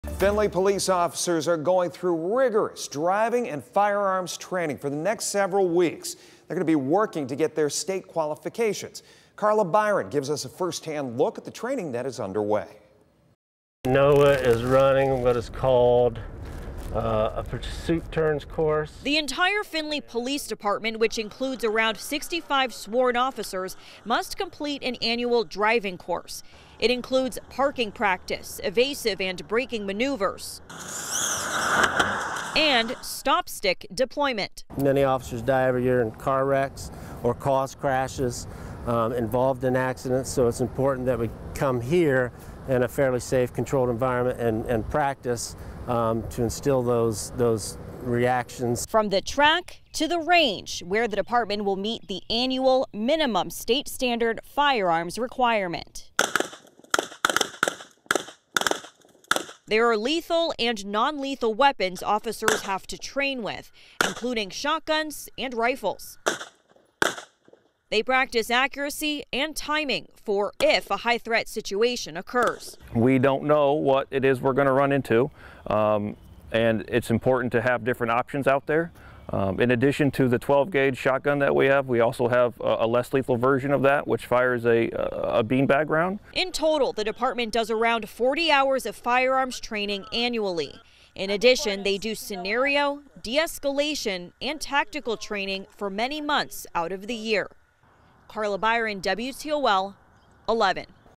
Findlay police officers participate in annual driving and firearms training
The Findlay Police Department is putting their officers through driving and firearms training. They'll each be working for the next several weeks to complete their annual state qualifications.

As part of the training, the entire department, which includes around 65 sworn officers, must complete a driving course at the Findlay Airport. The course includes parking practice, evasive and breaking maneuvers and stop stick deployment.

The department must also meet the annual minimum state standard firearms requirement. There are lethal and non-lethal weapons officers have to train with, including shotguns with live rounds, as well as bean bag rounds, rifles, handguns and tasers.